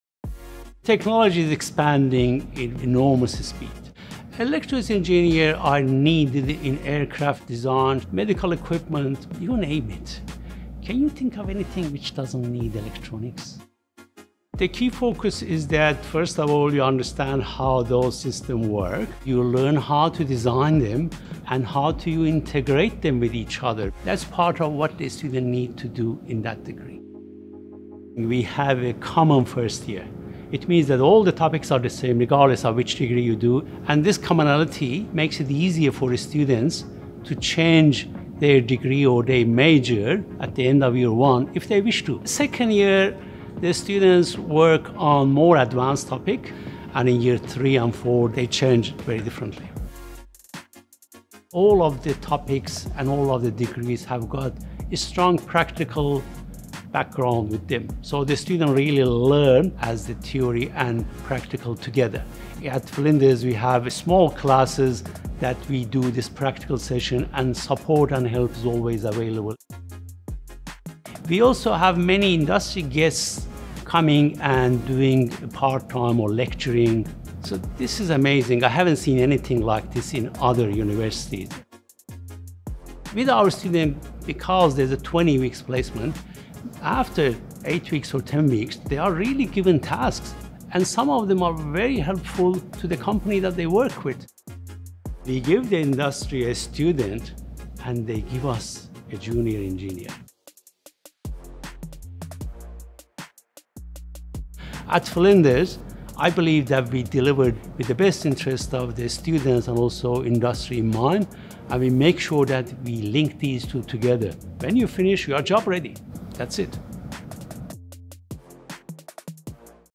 Study Flinders Electrical and Electronic Engineering
Meet Dr. Nasser Asgari, Director of Electrical and Electronic Engineering.

“Technology is expanding at an enormous speed. Electrical engineers are needed in air craft design, medical equipment you name it. Can you think of anything which doesn’t need electronics? The key focus is that first of all you understand how those systems work, you learn how to design them and how to integrate them with each other. That’s part of what the students need to do in that degree.”